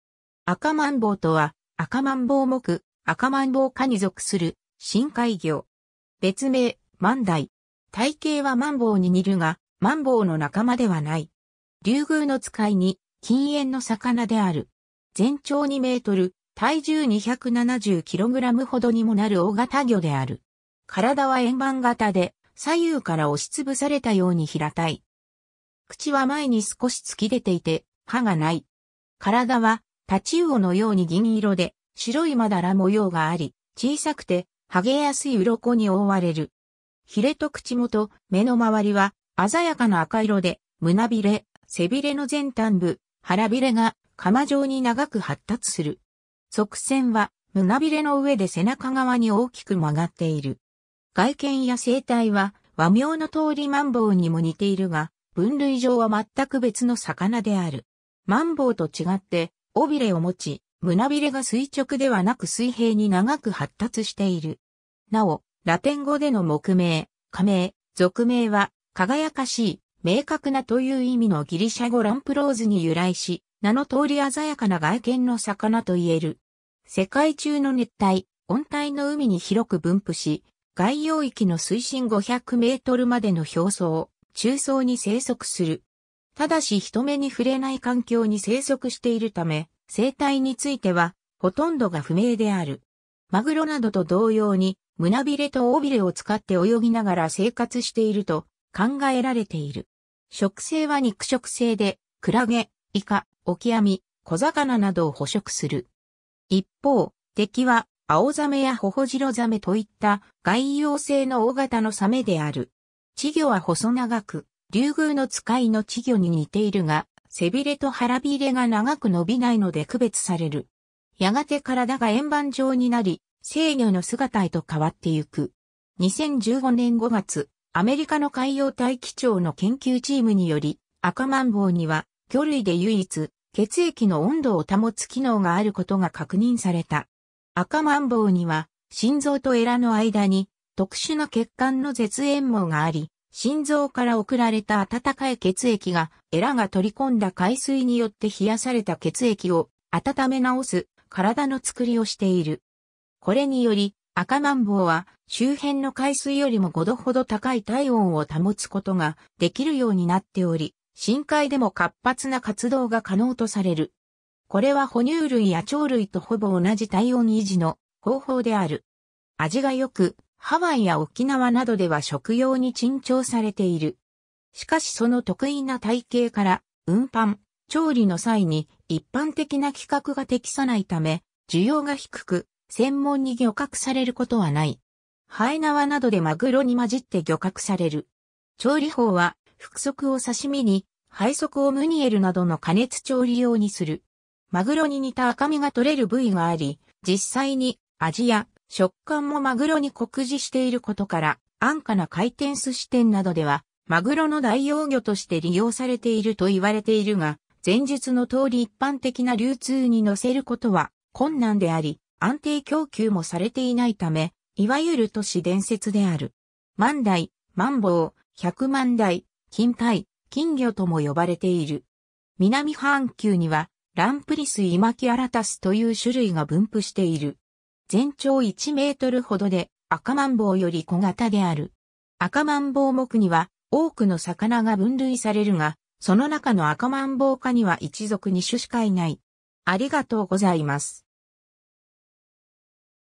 アカマンボウ
アカマンボウ（赤翻車魚、学名：Lampris guttatus、英名：opah）とは、アカマンボウ目アカマンボウ科 (Lampridae) に属する深海魚。
別名、マンダイ。
体型はマンボウ (Mola mola) に似るが、マンボウの仲間ではない。
リュウグウノツカイに近縁の魚である。
 全長2 m、体重270 kgほどにもなる大型魚である。
体は円盤形で、左右から押しつぶされたように平たい。
口は前に少し突き出ていて、歯がない。
体はタチウオのように銀色で、白いまだら模様があり、小さくて剥げやすい鱗に覆われる。
ひれと口元、目の周りは鮮やかな赤色で、胸びれ、背びれの前端部、腹びれが鎌状に長く発達する。
側線は胸びれの上で背中側に大きく曲がっている。
 外見や生態は和名のとおりマンボウにも似ているが、分類上はまったく別の魚である。
マンボウと違って尾びれをもち、胸びれが垂直ではなく水平に長く発達している。
なお、ラテン語での目名、科名、属名は、「輝かしい」「明確な」という意味のギリシャ語 lampros に由来し、名のとおり鮮やかな外見の魚といえる。
 世界中の熱帯・温帯の海に広く分布し、外洋域の水深500 mまでの表層・中層に生息する。
ただし人目に触れない環境に生息しているため、生態についてはほとんどが不明である。
 マグロなどと同様に、胸びれと尾びれを使って泳ぎながら生活していると考えられている。
食性は肉食性で、クラゲ、イカ、オキアミ、小魚などを捕食する。
いっぽう、敵はアオザメやホホジロザメといった外洋性の大型のサメである。
 稚魚は細長く、リュウグウノツカイの稚魚に似ているが、背びれと腹びれが長く伸びないので区別される。
やがて体が円盤状になり、成魚の姿へと変わってゆく。
 2015年5月、アメリカの海洋大気庁の研究チームにより、アカマンボウには魚類で唯一、血液の温度を保つ機能があることが確認された。
アカマンボウには、心臓とえらの間に特殊な血管の絶縁網があり、心臓から送られた温かい血液が、えらが取り込んだ海水によって冷やされた血液を温めなおす体の作りをしている。
これにより、アカマンボウは周辺の海水よりも5度ほど高い体温を保つことができるようになっており、深海でも活発な活動が可能とされる。
これは哺乳類や鳥類とほぼ同じ体温維持の方法である。
 味がよく、ハワイや沖縄などでは食用に珍重されている。
しかしその特異な体型から、運搬・調理の際に一般的な規格（発泡スチロールやまな板の大きさなど）が適さないため、需要が低く、専門に漁獲されることはない。
延縄などでマグロに混じって漁獲される。
 調理法は、腹側を刺身に、背側をムニエルなどの加熱調理用にする。
 マグロに似た赤身が取れる部位があり、実際に味や食感もマグロに酷似していることから、「安価な回転寿司店などではマグロの代用魚として利用されている」と言われているが、前述の通り一般的な流通に乗せることは困難であり、安定供給もされていないため、所謂都市伝説である。
 マンダイ、マンボウ、ヒャクマンダイ、キンタイ、金魚とも呼ばれている。
 南半球には Lampris immaculatus（英名：southern opah）という種類が分布している。
全長1 mほどで、アカマンボウより小型である。
 アカマンボウ目には多くの魚が分類されるが、その中のアカマンボウ科には1属2種しかいない。